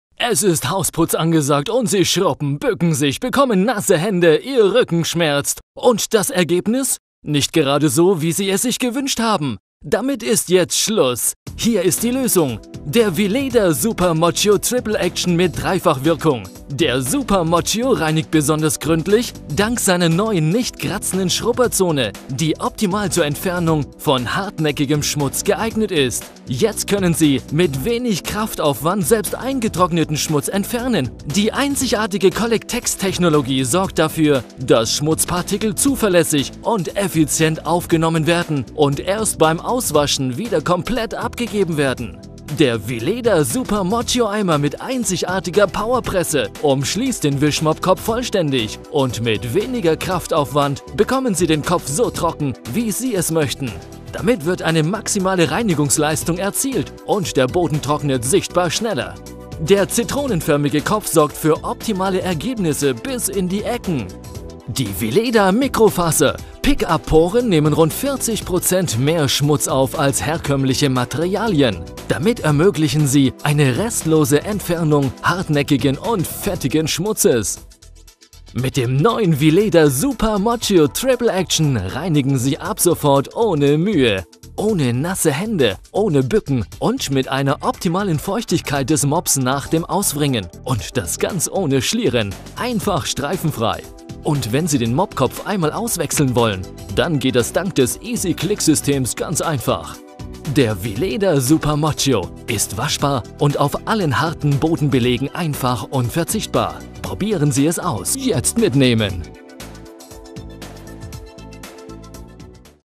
VILEDA Bodenwischer Super-Mocio 3-Action - jetzt bei ZGONC
Der VILEDA Bodenwischer ist ausgerüstet mit einem Easy-Click-System für leichteres Auswechseln des Wischkopfes. Die blauen Mikrofaserstreifen sind speziell ausgearbeitet für die Aufnahme von fetthaltigen Schmutz und Flecken.

Jetzt bei ZGONC, dem echten Fachgeschäft Österreichs und in unserem Online Shop.